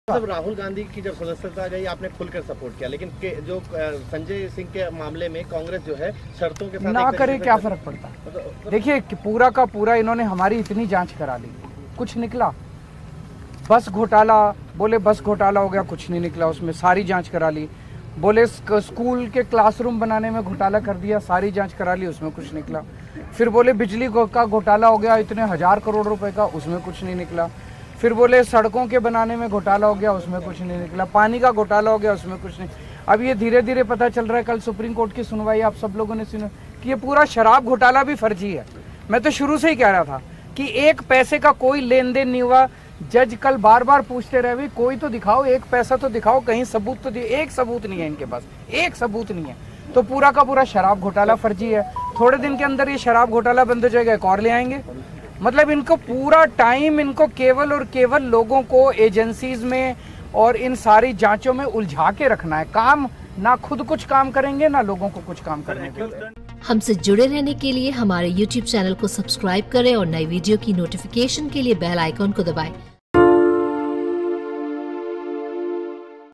ED की जांच पर Kejriwal के सवाल- ‘सारी जांच करा ली लेकिन कुछ नहीं निकला, पूरा शराब घोटाला ही फर्जी’
शराब घोटाले में ED की जांच पर केजरीवाल ने उठाए सवाल
जांच एजेंसी के पास सबूत हैं तो वो उन्हें सबके सामने रखें: अरविंद केजरीवाल
‘सारी जांच करा ली लेकिन कुछ नहीं निकला, पूरा शराब घोटाला ही फर्जी है’
‘बिजनेस, व्यापार, इंडस्ट्री में डर का माहौल, यह देश के लिए अच्छा नहीं है’